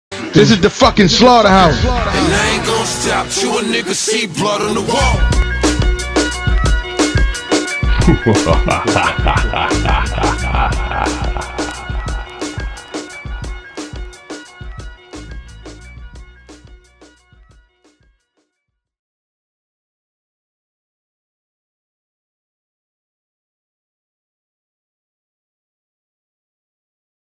JOE BUDDEN vs HOLLOW DA DON (comment/share)
Leave your comments/share for this battle in cartoon prediction form before TOTAL SLAUGHTER.